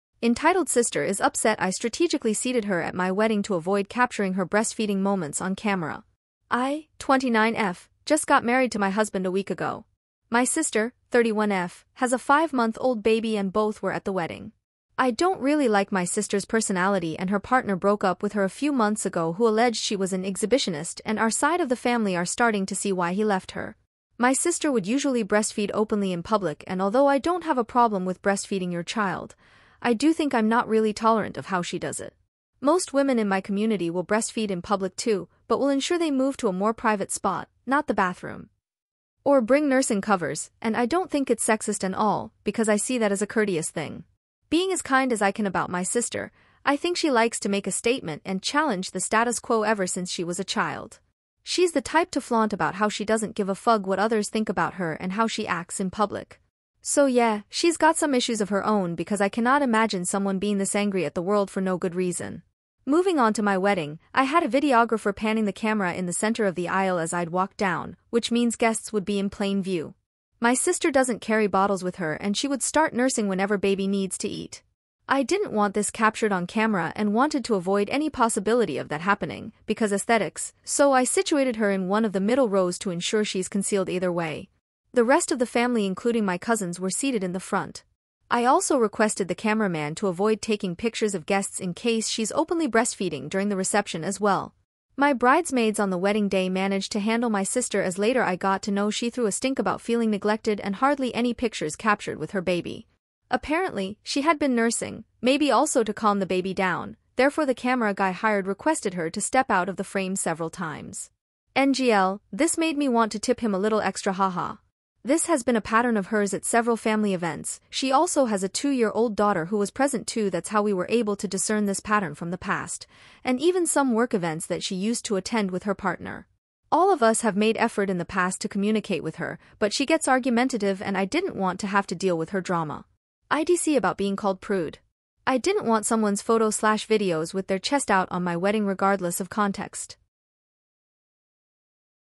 Entitled Sister is Upset I Strategically Seated her at My Wedding to Avoid Capturing her Bre@stfe*d
Entitled sister is upset I strategically seated her at my wedding to avoid capturing her bre@stfeeding moments on camera